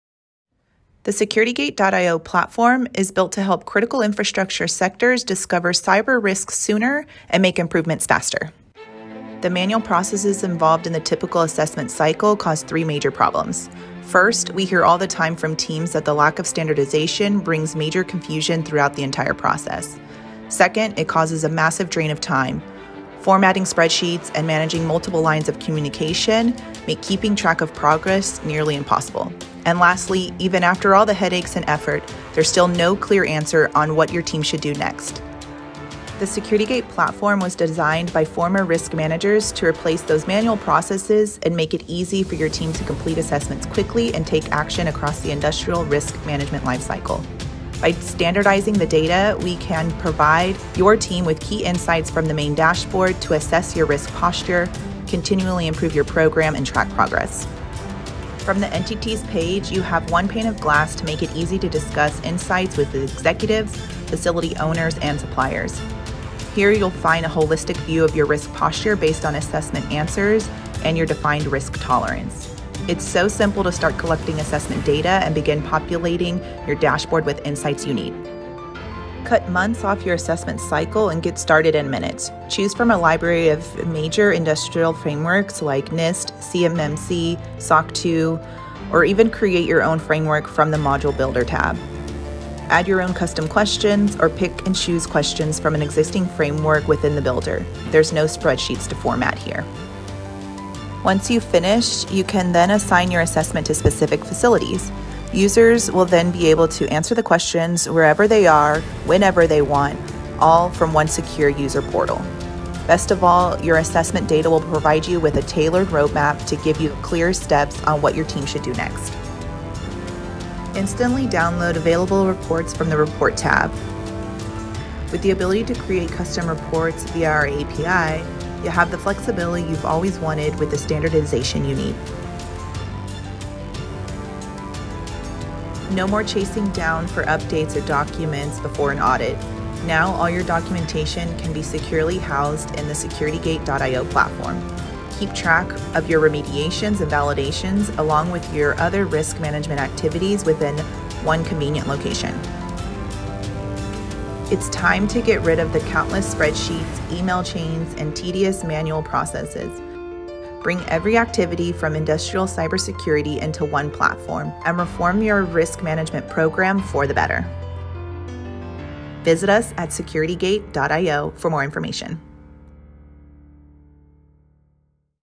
SecurityGate.io Platform Tour
The SecurityGate.io platform is built to help critical infrastructure sectors discover cyber risks sooner and make improvements faster!

The manual processes involved in the typical assessment cycle cause 3 major problems:

First, we hear all the time from teams that the lack of standardization brings major confusion throughout the entire process.

Second, it causes a massive drain of time. Formatting spreadsheets and managing multiple lines of communication make keeping track of progress nearly impossible.

And lastly, even after all the headaches and effort, there’s still no clear answer on what your team should do next

The SG platform was designed by former risk managers to replace those manual processes and make it easy for your team to complete assessments quickly and take action across the industrial risk management lifecycle.

By standardizing the data, we are able to provide your team with key insights from the main dashboard to asses your risk posture, continually improve your program, and track progress.

Entity Dashboard

From the Entities page, you have one pane of glass to make it easy to discuss insights with executives, facility owners, and suppliers.

Here you’ll find a holistic view of your risk posture based on assessment answers and your defined risk tolerance.

It’s so simple to start collecting assessment data and begin populating your dashboard with the insights you need.

Assessment Creation

Cut months off your assessment cycle and get started in minutes. Choose from a library of major industrial frameworks like NIST, CMMC, SOC 2, etc. or even create your own framework from the Module Builder tab.

Add your own custom questions or pick and choose questions from existing frameworks within the builder. There’s no spreadsheets to format here!

Once you’ve finished, you can then assign your assessment to specific facilities. Users will then be able to answer the questions wherever they are, whenever they want to, all from one secure User portal. Best of all, your assessment data will provide you with a tailored roadmap to give you clear steps on what your team should do next.

Reporting

Instantly download compliance reports from the Report tab. With the ability to create custom reports via our API, you have the flexibility you’ve always wanted with the standardization you need.

No more chasing down people for updates or documents before an audit. Now, all your documentation can be securely housed inside The SecurityGate.io platform. Keep track of your remediations and validations, along with your other risk management activities within one convenient location.

It’s time to get rid of the countless spreadsheets, email chains, and tedious manual processes. Bring every activity for industrial cybersecurity into one platform, and reform your risk management program for the better.

Attributions:
Footage/B-roll by Videezy